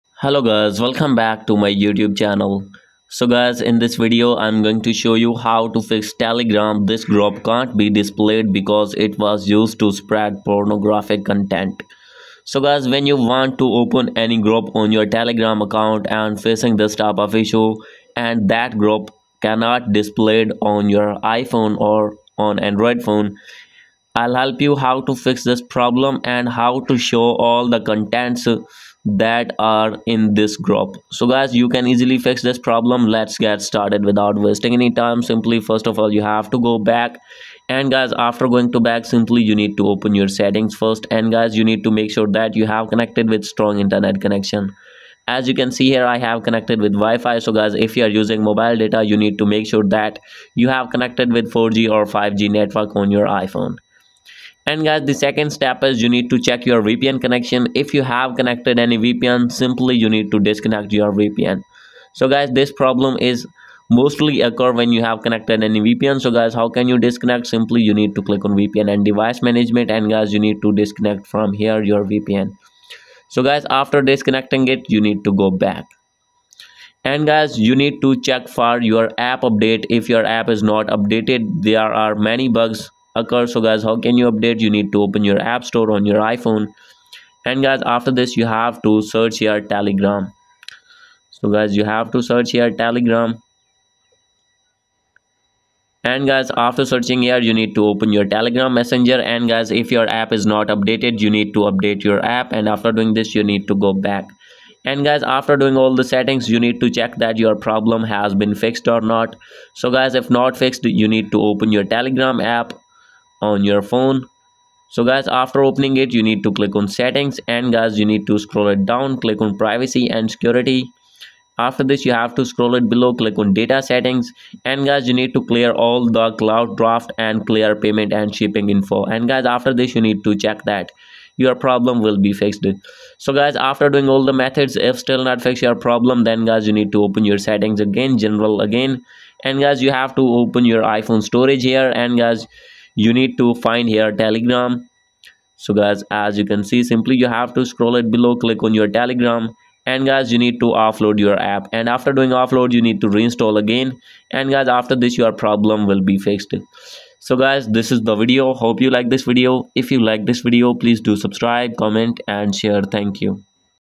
Fixed: Telegram This Group Cannot be Displayed Because it Was Used to Spread iPhone 2024
In this video, I will help you to fix telegram this group cannot be displayed because it was used to spread iphone. Please watch till the end.